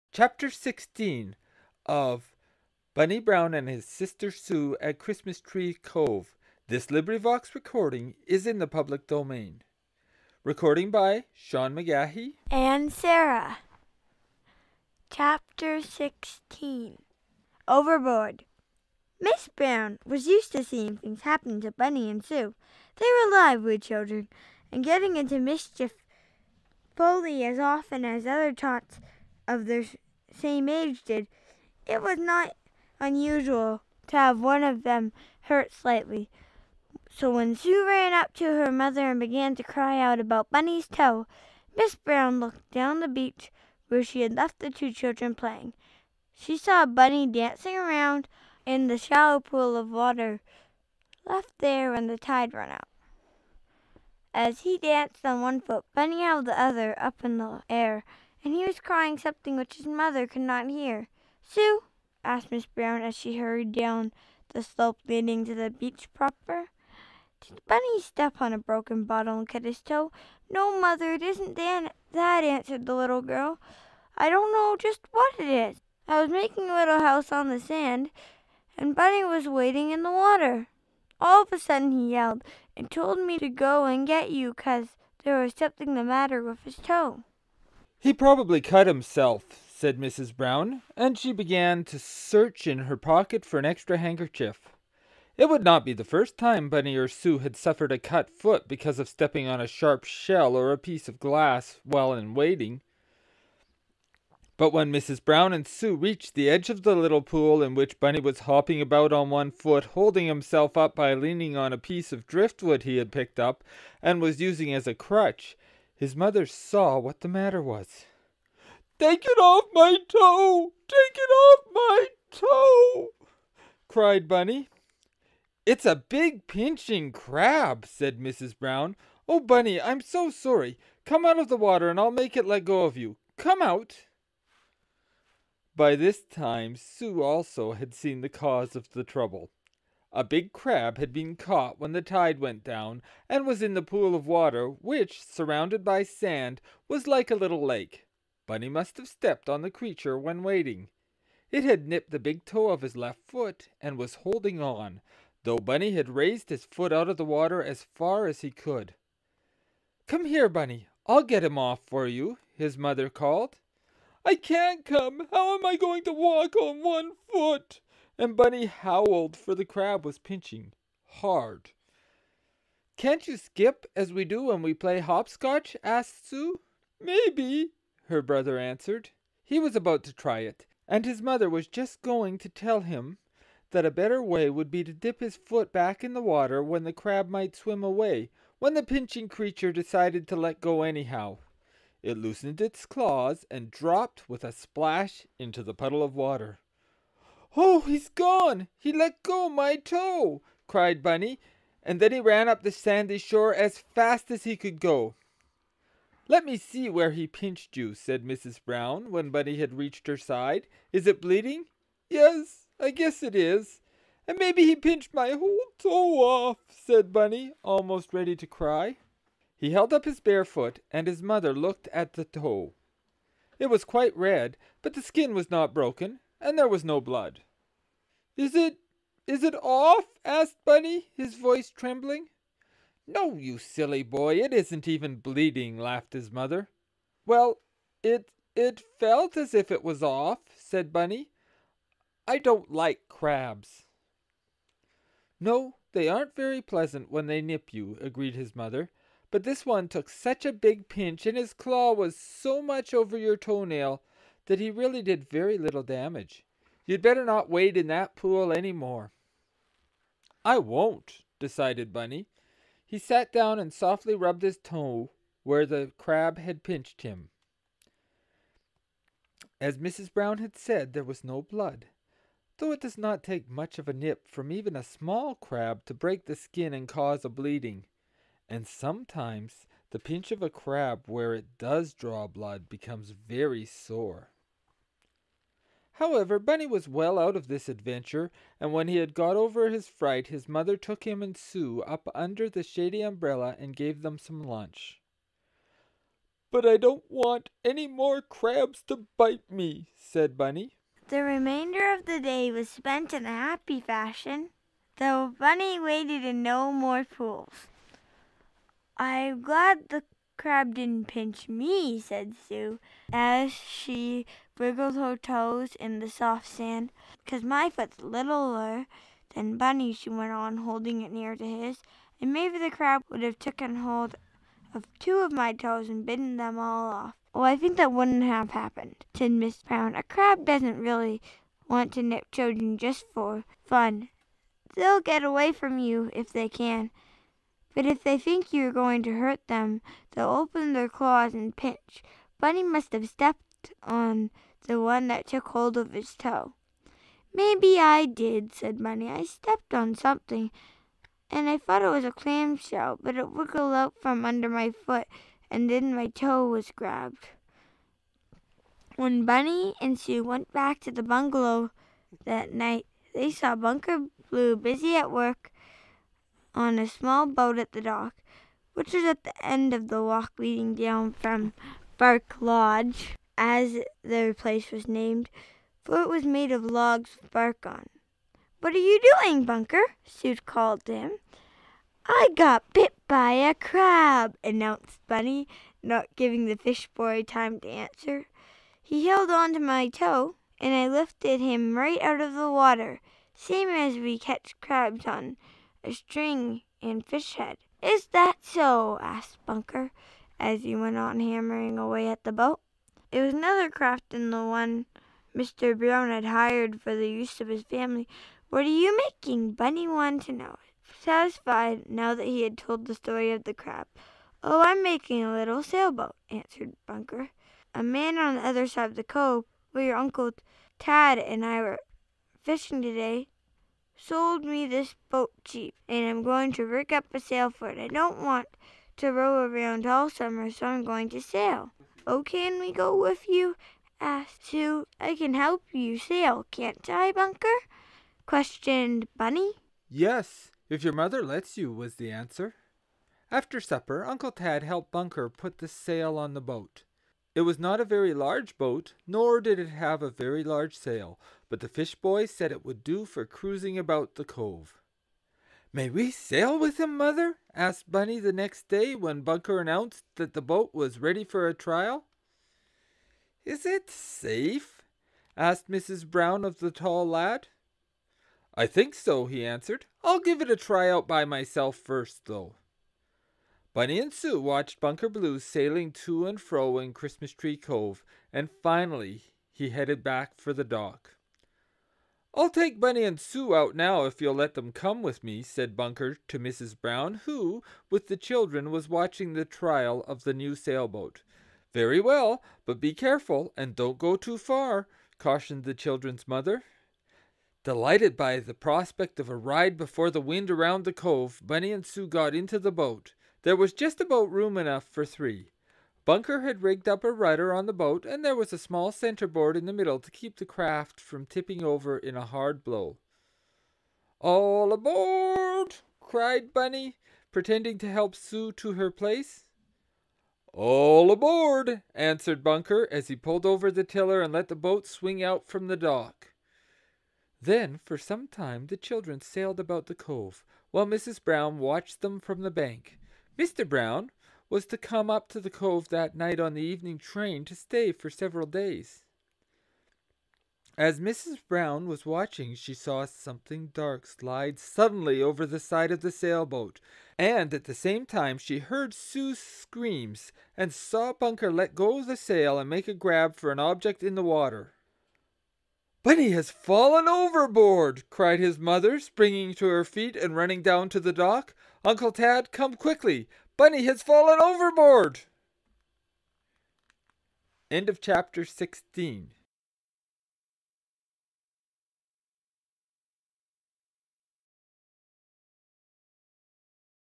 Bunny Brown and his Sister Sue at Christmas Tree Cove 16 by Laura Lee Hope Full Audiobook
Bunny Brown and his Sister Sue at Christmas Tree Cove
Laura Lee Hope

Bunny Brown and His Sister Sue were featured in a series of 20 books for young children published by the Stratemeyer Syndicate from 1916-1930. In this adventure, first published in 1920, Bunny and Sue lose a valuable possession belonging to their mother. They have many adventures and misadventures during a family boating vacation to Christmas Tree Cove. (Introduction by S. McGaughey)

Read in duet by Rose 264 and Sean McGaughey.

Genre(s): Family

Language: English